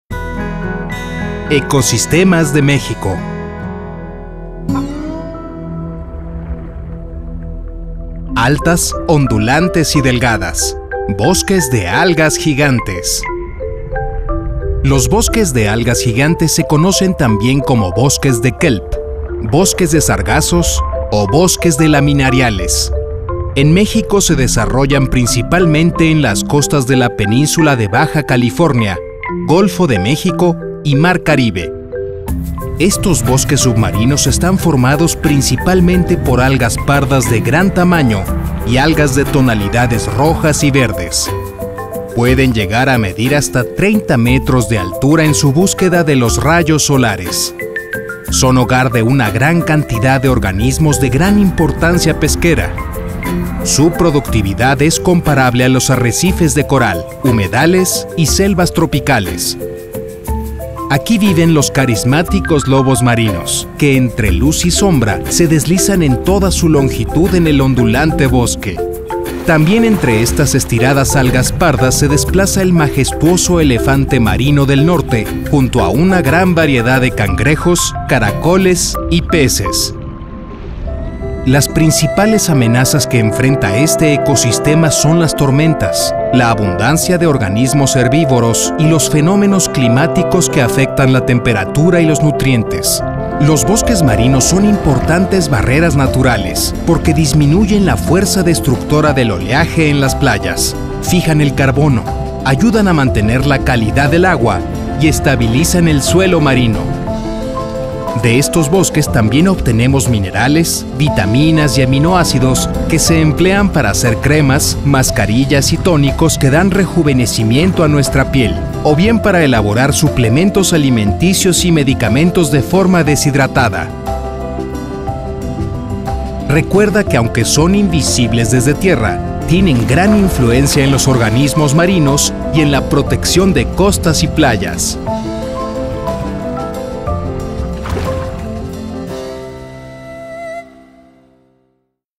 Altas, ondulantes y delgadas: Bosques de algas gigantes - ECOSISTEMAS DE MÉXICO
Los bosques de algas gigantes se conocen también como bosques de kelp, bosques de sargazos  o bosques de laminariales.
En México se desarrollan principalmente en las costas de la península de Baja California, Golfo de México y Mar Caribe.